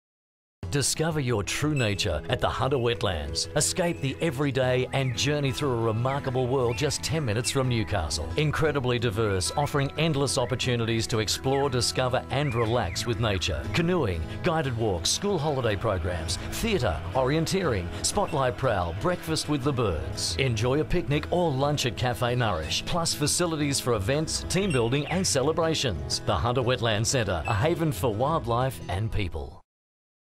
NB8080830 Hunter Wetlands.wmv
NBN TV ad for Hunter Wetlands Centre part of the Hunter Estuary Ramsar Site